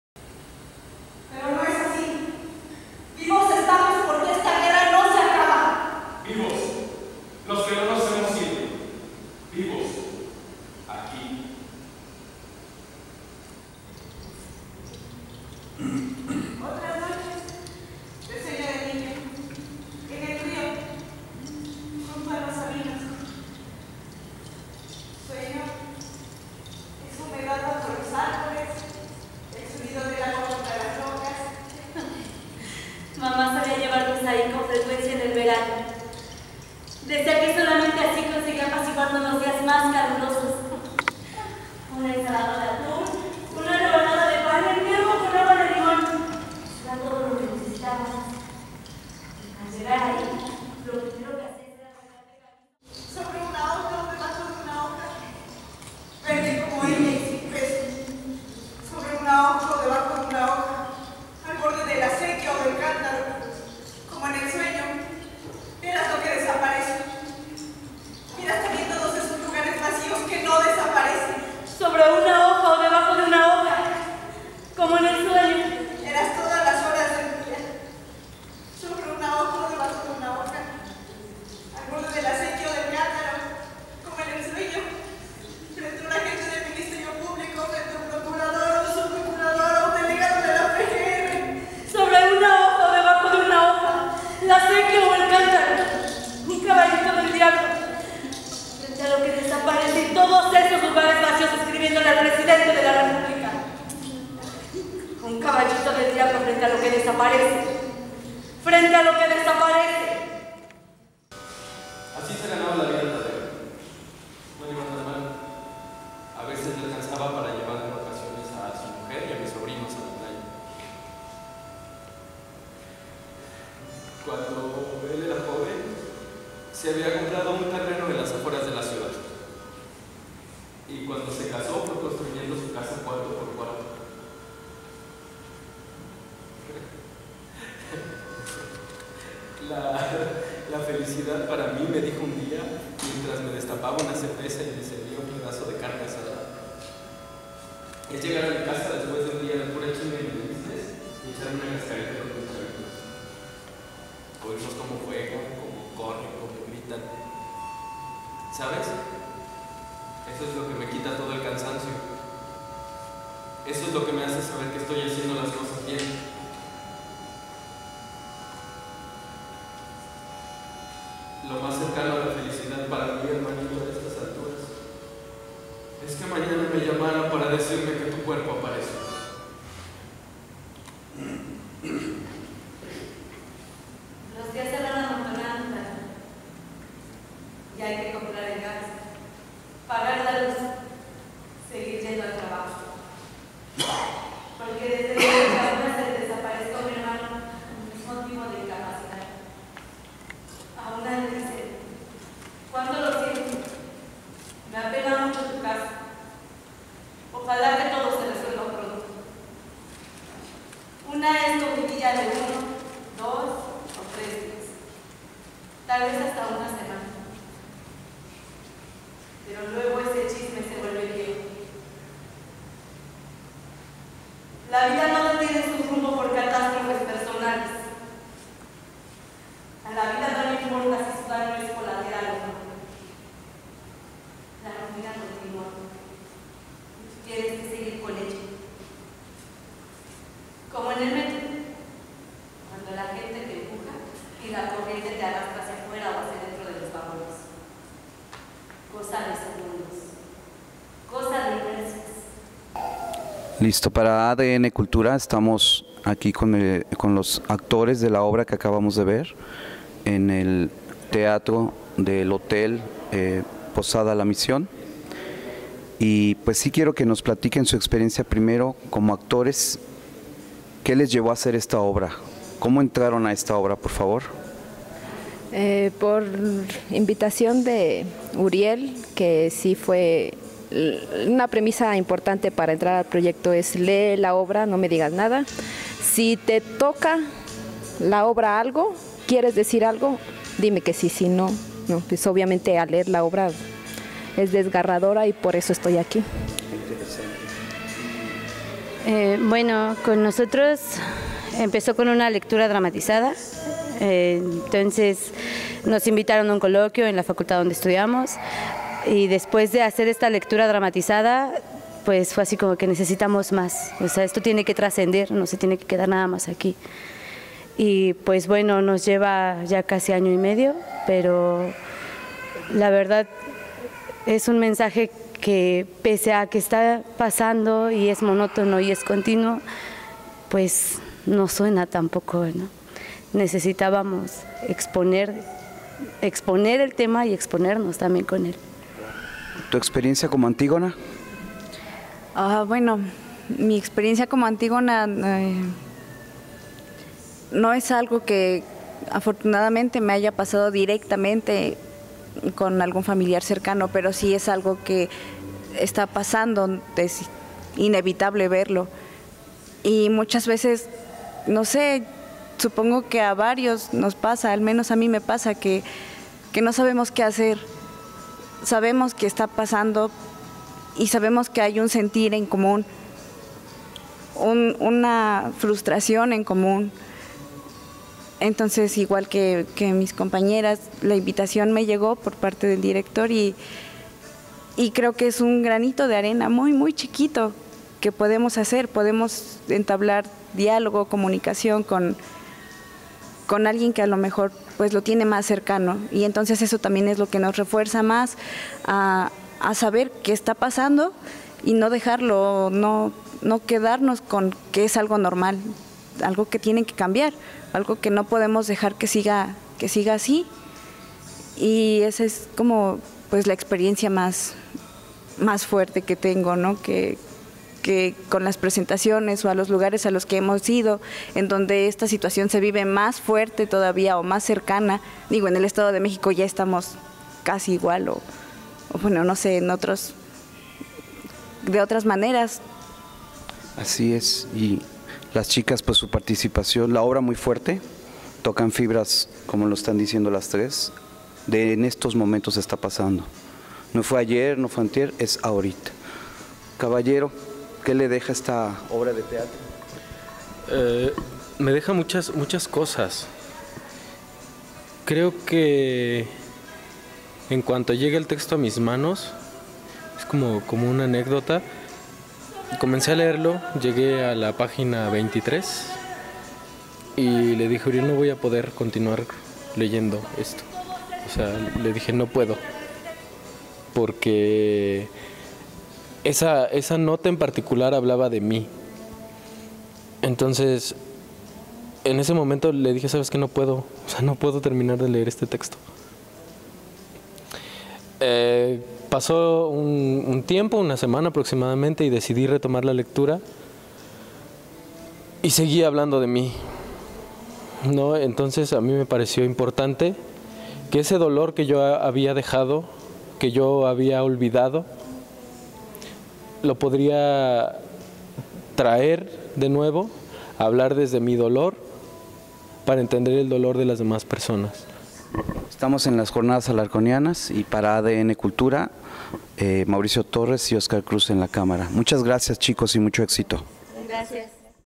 Obra de teatro Antígona González
Obra de teatro Antígona González fue presentada en las XXX Jornadas Alarconianas